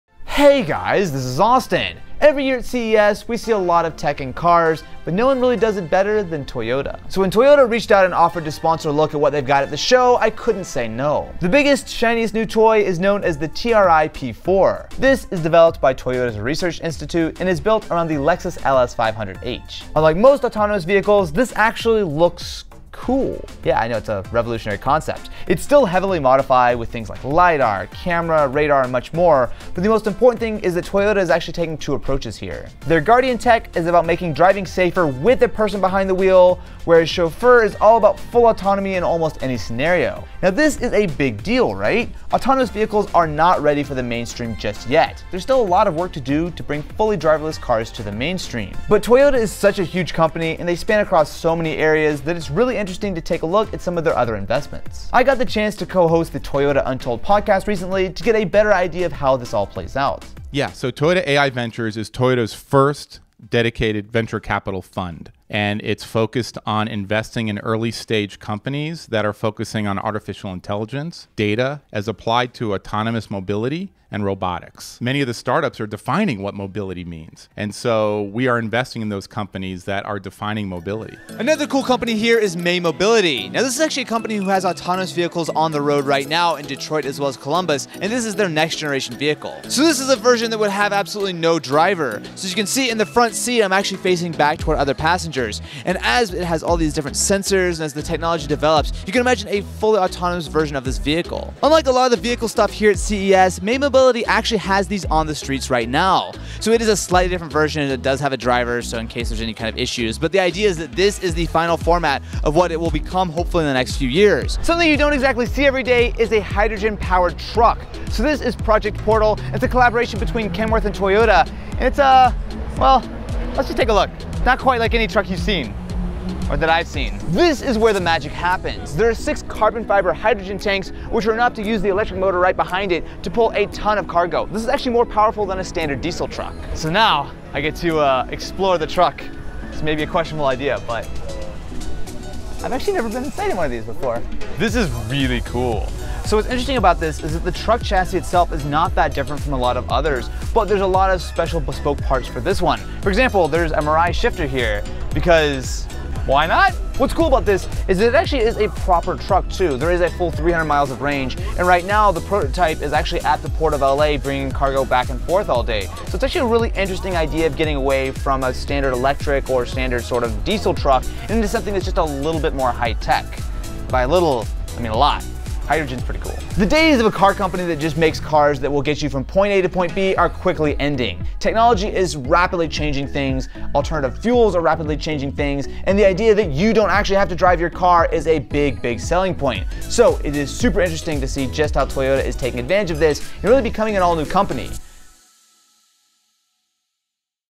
Toyota's Car of the Future
At CES 2019 Toyota gave a glimpse at the future of the car industry.